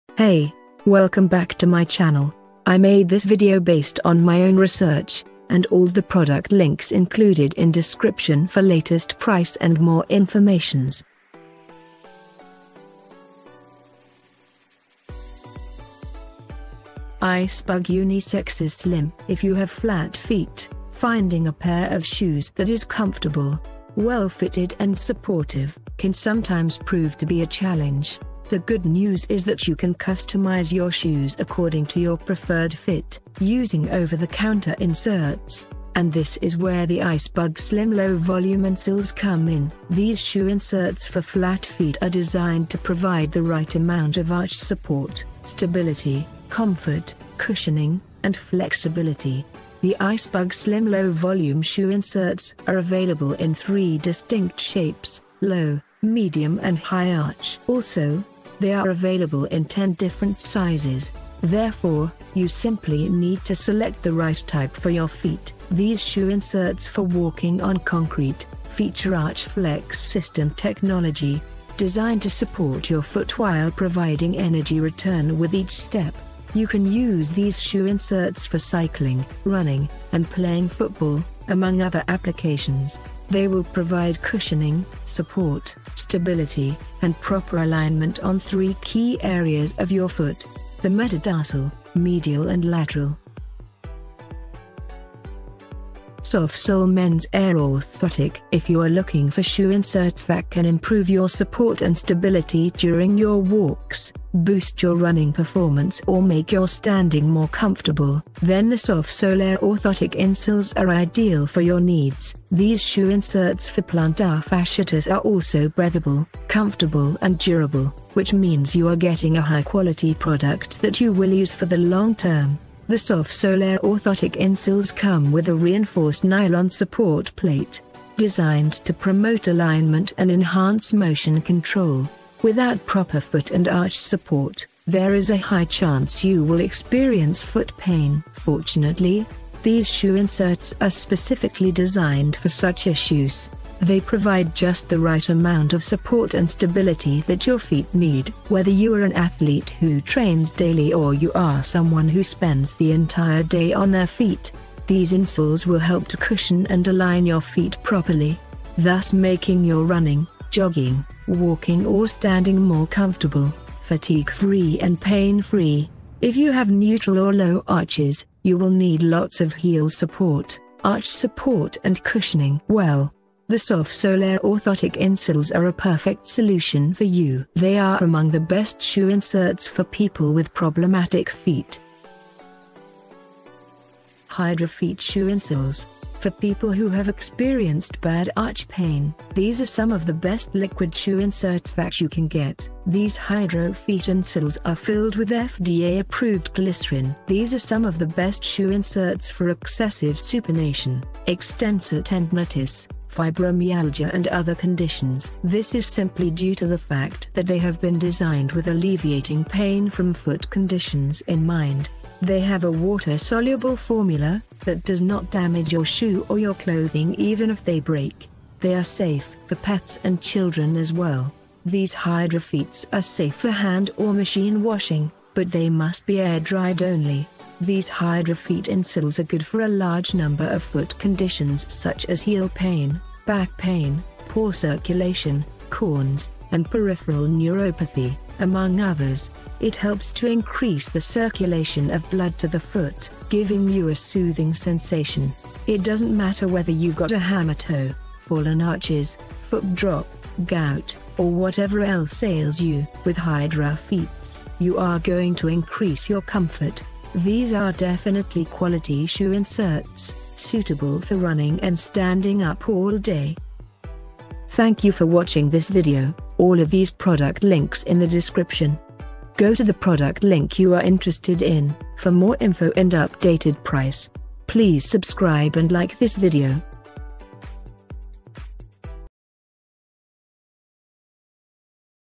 ✅ Top 3 Best Shoe Inserts Reviews 2022?
► Here is the Links For Updated Price Of Best Shoe Inserts 2022!
✓ 1. Hydrofeet Shoe Insoles ▶️
✓ 2. RooRuns 3/4 Length Arch ▶️
✓ 3. Sof Sole Men's Airr Orthotic ▶️

This is my top 3 Best Shoe Inserts, That I mention in this video.